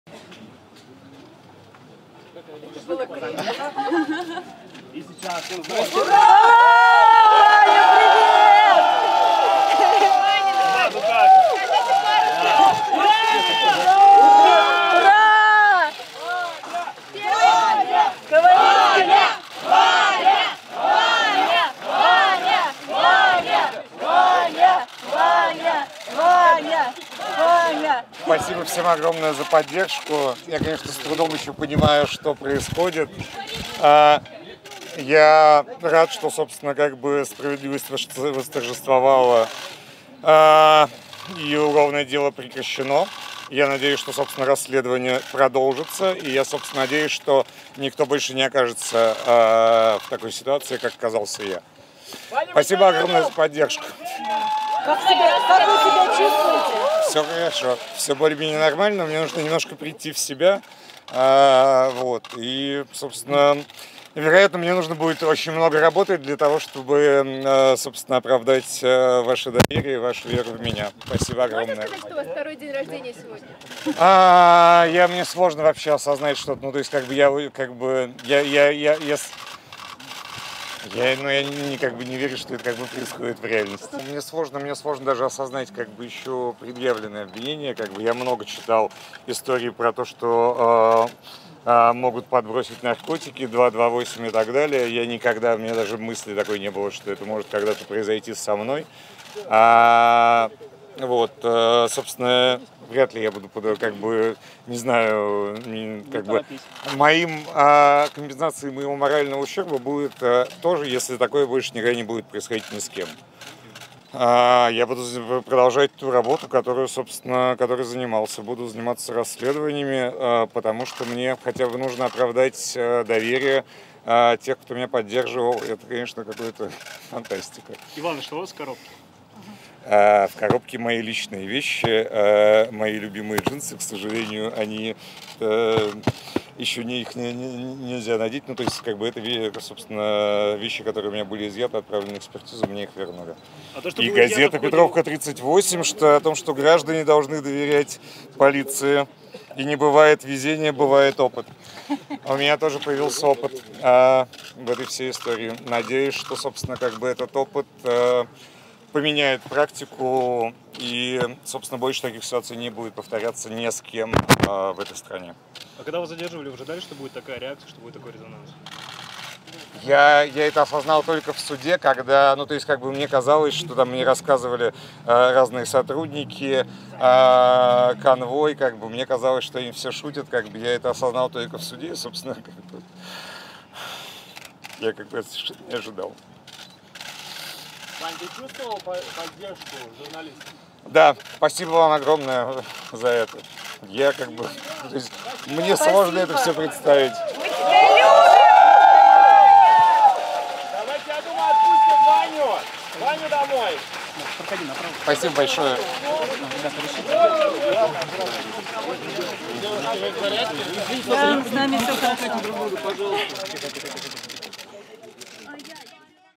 Комментарий Ивана Голунова после освобождения
Сегодня было прекращено уголовное дело в отношении журналиста «Медузы» Ивана Голунова. 7 июня он был арестован по сфальсифицированному обвинению в сбыте и хранении наркотиков. Иван известен как автор громких расследований о коррупции в московской мэрии.

Освобождение Ивана — большая победа людей, которые выступили в его поддержку. Но важно помнить, что тысячи невиновных людей продолжают сидеть по сфабрикованным обвинениям. В том числе Леонид Волков, которого уже второй раз отправили под арест за трансляцию митинга против повышения пенсионного возраста.

Монтажер — Кирилл Семенов
Продюсер —